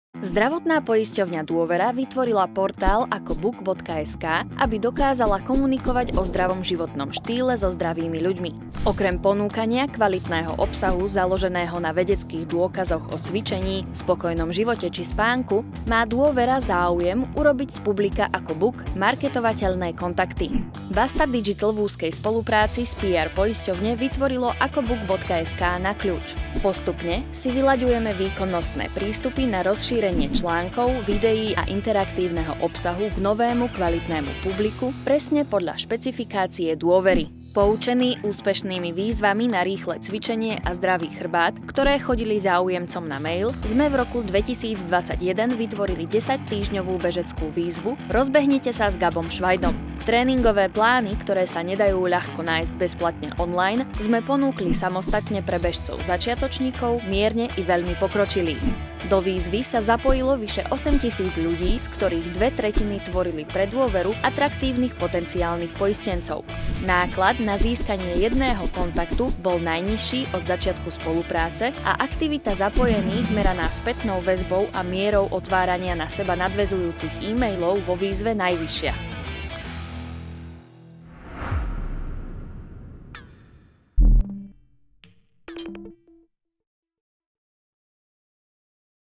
8000+ zdravých ako buk
Do výzvy sa zapojilo vyše 8 000 ľudí, z ktorých dve tretiny tvorili pre Dôveru atraktívnych potenciálnych poistencov. Náklad na získanie jedného kontaktu bol najnižší od začiatku spolupráce a aktivita zapojených meraná spätnou väzbou a mierou otvárania na seba nadväzujúcich emailov vo výzve najvyššia.